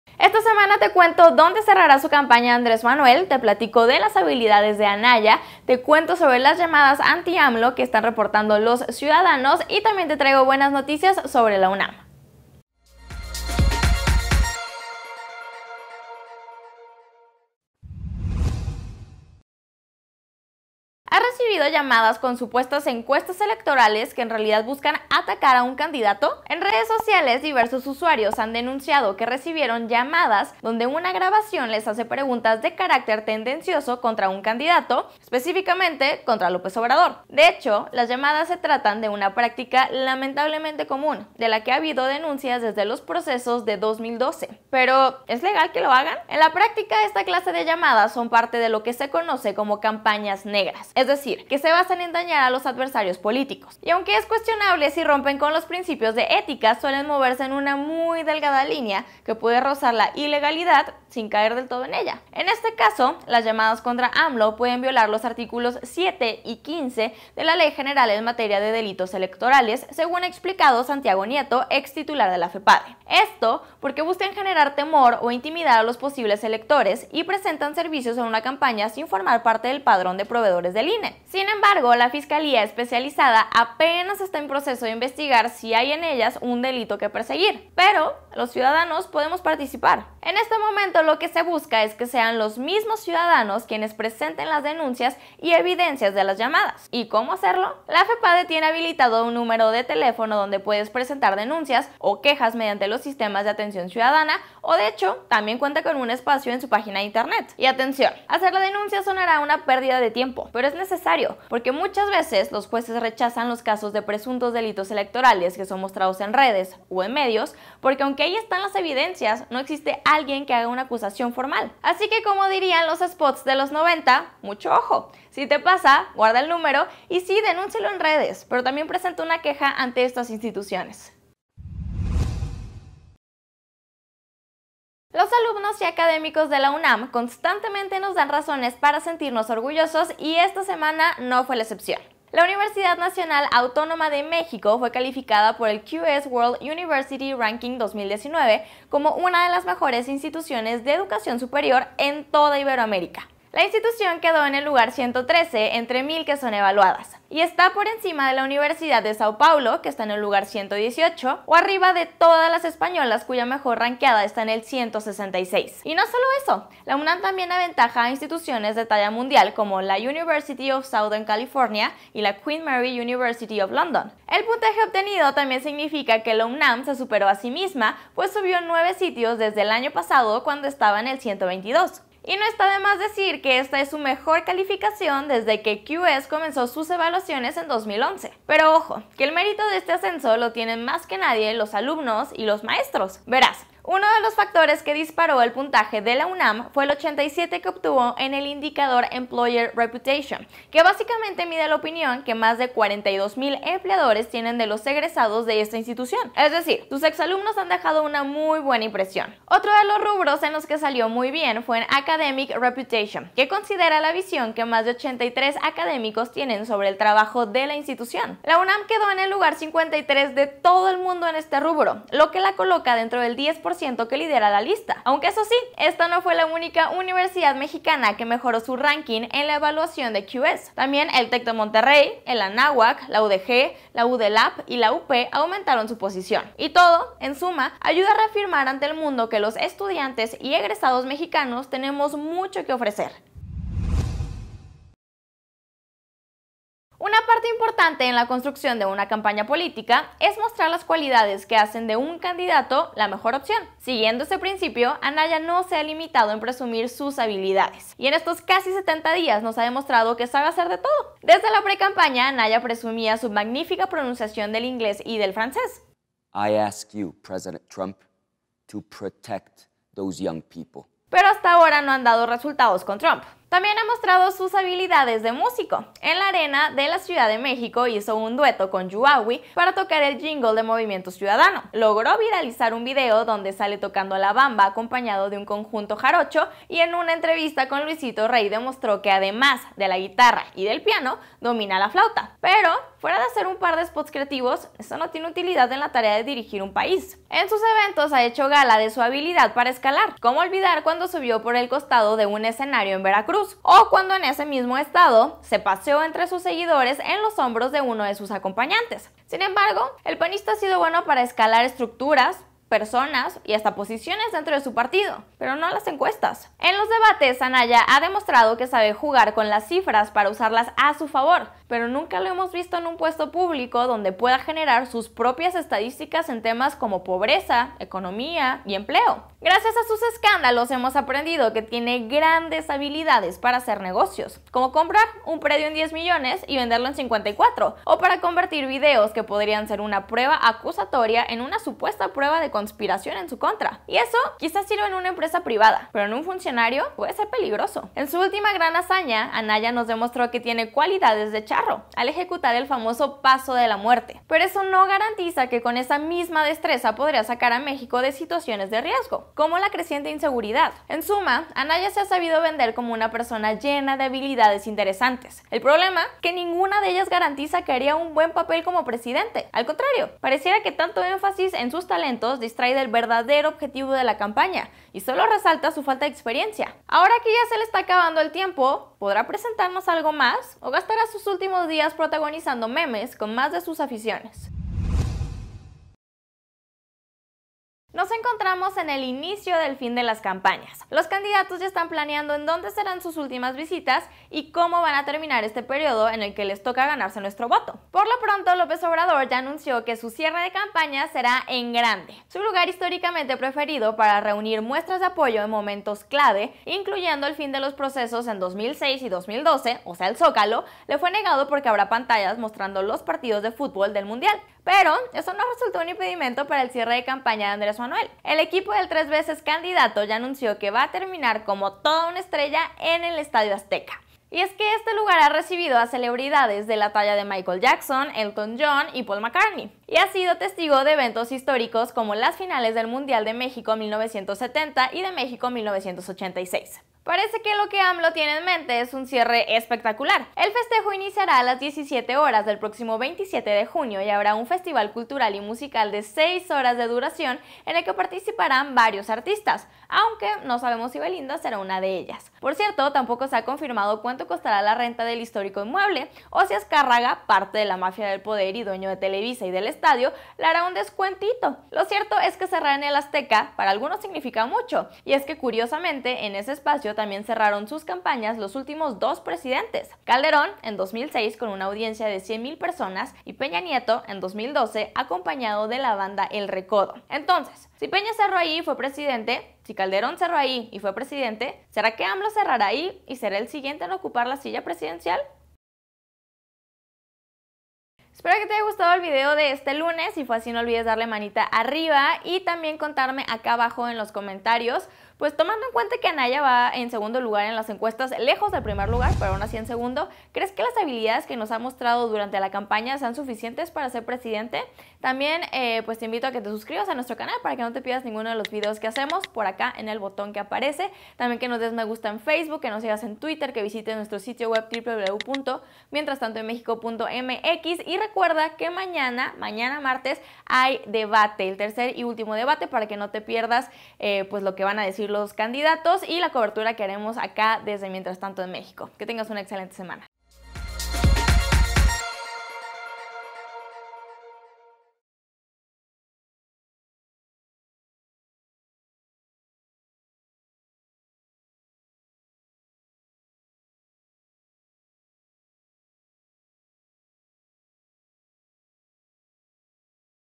Anaya, el músico y charro que quiere ser presidente I Mientras Tanto en México
Esta semana hacemos un recuento de la campaña de Anaya, te decimos qué hacer si recibiste una llamada anti AMLO, te contamos una buena noticia sobre la UNAM y te platicamos donde será el cierre de campaña de López Obrador.